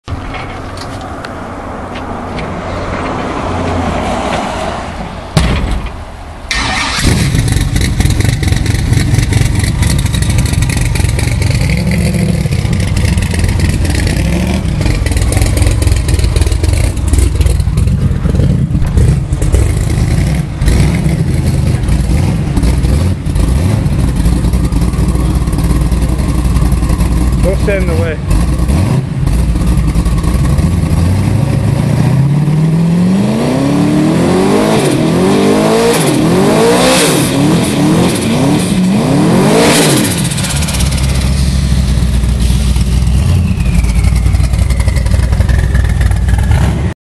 my 77 camaro twin turbo
406 sbc twin 88mm turbos non tuned test burnout